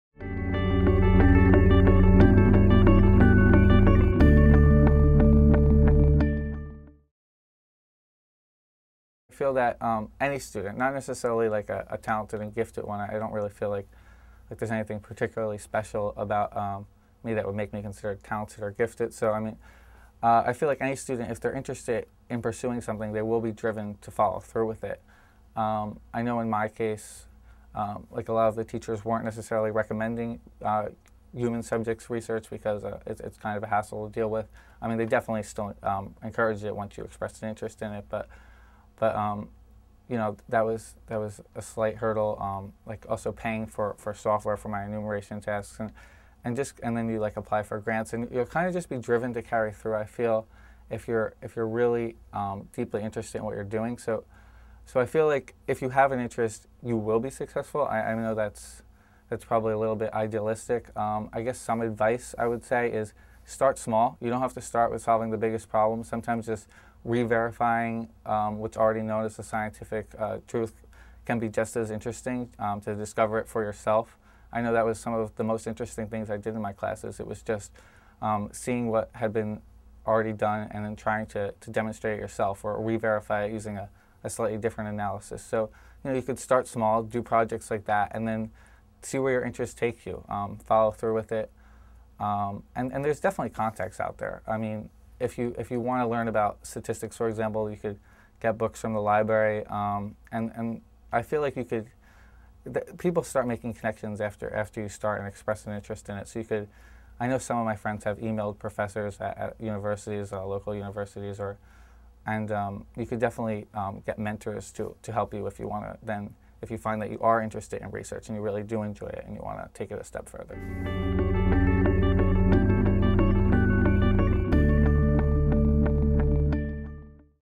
Steven Castellano Considers Talented and Gifted Education | Big Think
Steven Castellano Considers Talented and Gifted Education
If you have a passion, you can achieve great things, whether you are gifted or not.
Steven Castellano:

Big Think extends a warm thank you to Cogito and the Center for Talented Youth at Johns Hopkins University for facilitating this interview.

Steven Castellano is a senior at High Technology High School in Lincroft, New Jersey, and is one of Big Think's "10 under 25" young experts.

At High Tech, he was the vice-president of the Key Club, vice-president of the National Honor Society, and a member of the senior class council. He also served as the secretary for the New Jersey state Technology Student Association that works to promote technological literacy in schools throughout the country. He has developed a deep interest in behavioral neuroscience research throughout high school and has worked on projects investigating the effects of acupressure on alertness and visual attention skills.

His research has won first prize in the MIT THINK competition, the Delaware Valley Science Fair, the Jersey Shore Science Fair, and the New Jersey Academy of Science. In addition, Steven has been named an Intel Science Talent Search semifinalist and has presented his research to the American Junior Academy of Science division of the American Association for the Advancement of Science, the Human Factors and Ergonomics division of the Federal Aviation Administration, the Institute of Electrical and Electronics Engineers, and the Armed Forces Communications Electronics Association. Most recently, his research has been published in Imagine magazine.

In 2009, Steven and his teammates won $20,000 in the 2009 Moody's Mega Math Challenge, an applied mathematics competition sponsored by the Moody's Foundation and the Society for Industrial and Applied Mathematics. During the challenge, his team had 14 hours to analyze and submit a paper on the American Recovery and Reinvestment Act of 2009. The team was also featured on Bloomberg Radio's Taking Stock with Pimm Fox and Karen Moscow. His paper and presentation can be viewed at 2009 Winning Papers.

After graduating from High Tech, Steven plans to attend Columbia College of Columbia University to study neuroscience and physics.
TRANSCRIPT:

Question: What's your advice for aspiring scientists?

Steven Castellano: I feel that any student, not necessarily like a talented and gifted one, I don't really feel like there's anything particularly special about me that would make me considered talented and gifted, so I mean, I feel like any student, if they are interested in pursuing something they will be driven to follow through with it. I know in my case, like a lot of the teachers weren't necessarily recommending human subjects research, because it's kind of hassle to deal with. I mean they definitely encouraged it once you expressed interest in it, but you know, that was a slight hurdle. Also paying for software for my Enumeration Task and then you like apply for grants and you'll kind of just be driven to carry through I feel, if you're really deeply interested in what you're doing, so I feel like if you have an interest, you will be successful. I know that's probably a little bit idealistic I guess some advice I would say is start small, you don't have to start with solving the biggest problems, sometimes just re-verifying what is already known as a scientific truth can be just as interesting, to discover it for yourself. I know that was some of the most interesting things I did in my classes, it was just seeing what have been already been done and then trying to demonstrate yourself or re-verify using a slightly different analysis. So you could start small, do projects like that, and then see where your interests take you. Follow through with it and there's definitely contacts out there, I mean, if you want to learn about statistics, for example, you could get books from the library. And I feel like people start making connections after you start and express interest in it. I know some of my friends have emailed professors at universities or local universities and you could definitely get mentors to help you, then if you find that you are interested in research and you really do enjoy it, and you want to take it a step further.